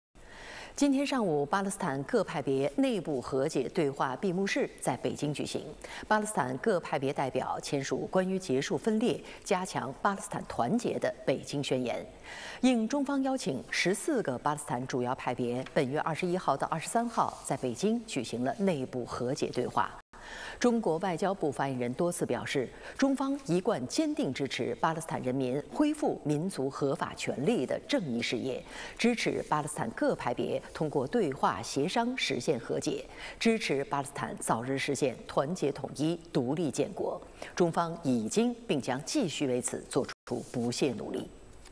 巴勒斯坦各派在北京签署关于结束分裂的《北京宣言》
7月21日-23日，巴勒斯坦各派别内部和解对话在北京举行。今天（23日）上午，巴勒斯坦各派别内部和解对话闭幕式举行。

巴勒斯坦各派代表签署关于结束分裂、加强巴勒斯坦团结的《北京宣言》。